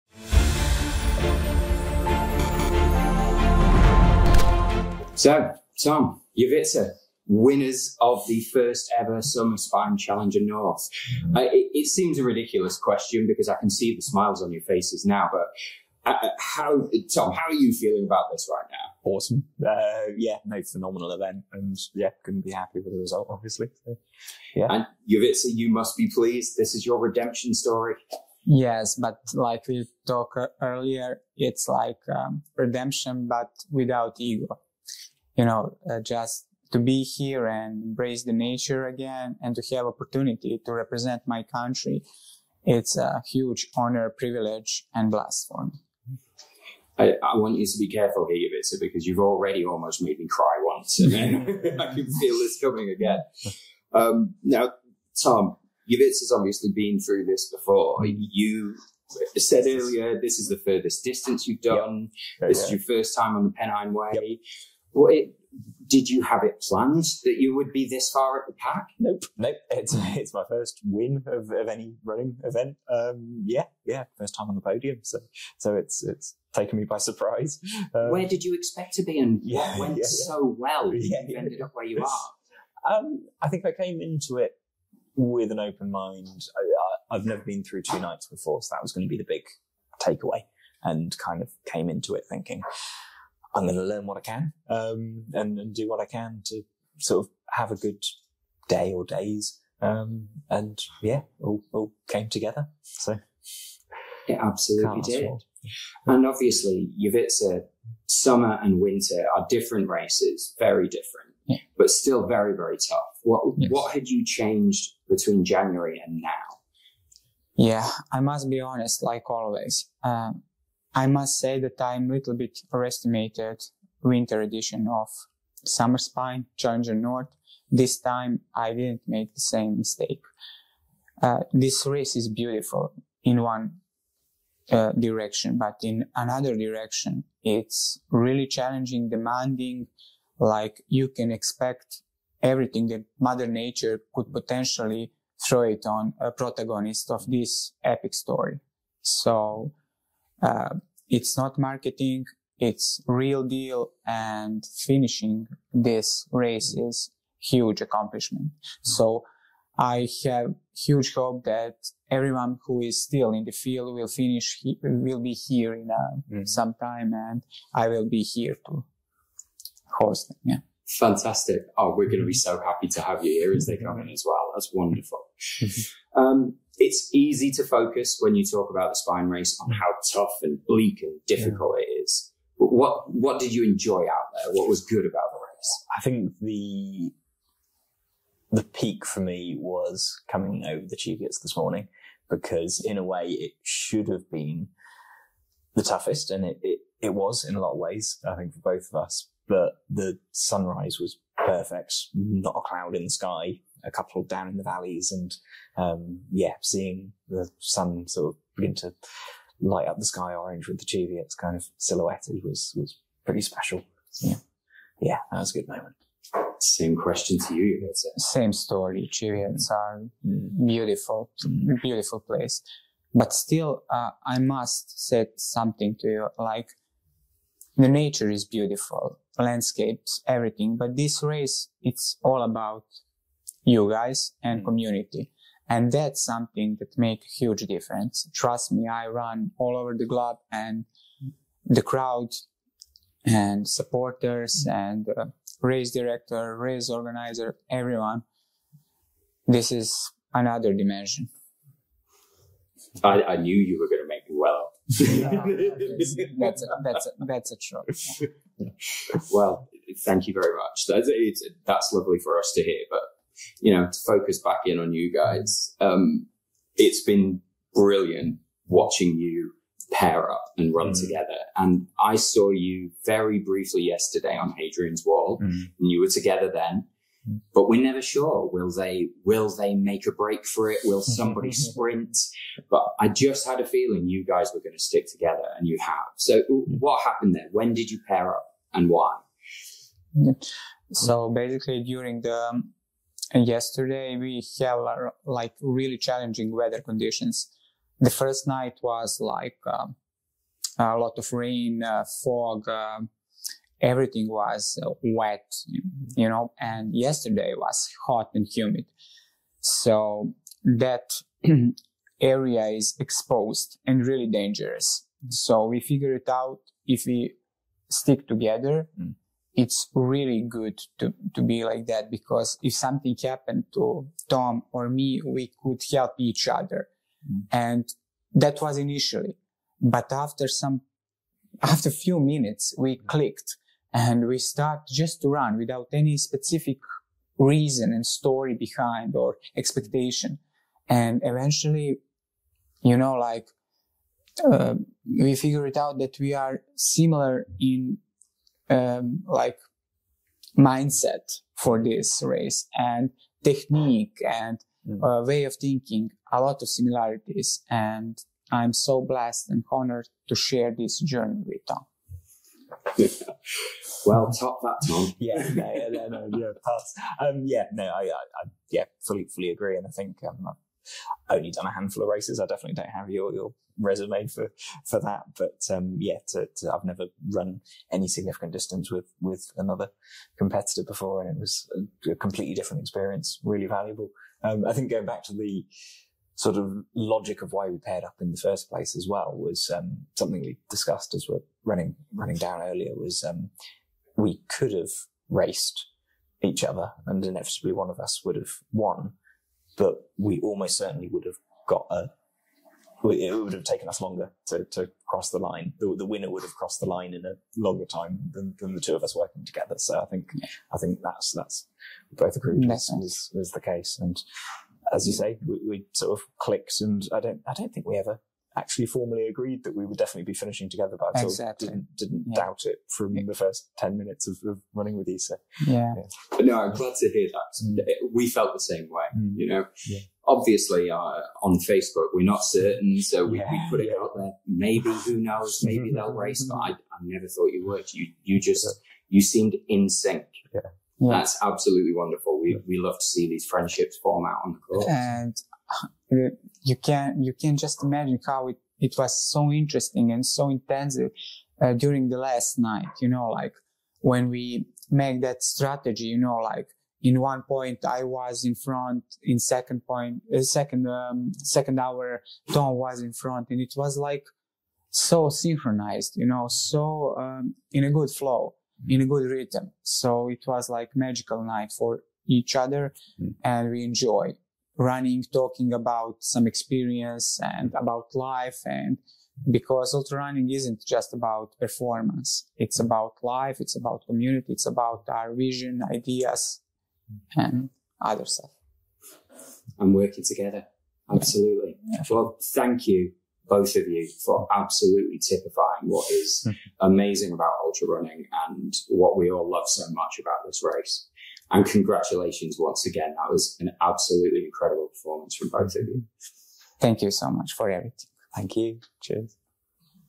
Montane Summer Spine 2023 | Winner Interviews | Jovica Spajic and Tom Hill
Shortly after becoming joint winners of the first-ever Montane Summer Spine Challenger North, Jovica Spajic and Tom Hill chatted with Will Roberts from the Spine Media Team about their time on the Pennine Way.

For Jovica, of Serbia, there was unfinished business as he was returning to this course after retiring from the Winter edition of this race back in January. For Tom, this was a voyage into the unknown having never covered this distance before.
We also hear about how they eventually formed a partnership and how that helped them achieve such an impressive win.

Entries for the Montane Summer Spine Races 2024 will open on Monday 10th July 2023.
Entries for the Summer Sprint open at 12:00 midday.
Entries for the Spine, Challenger North and Challenger South will follow at 13:00.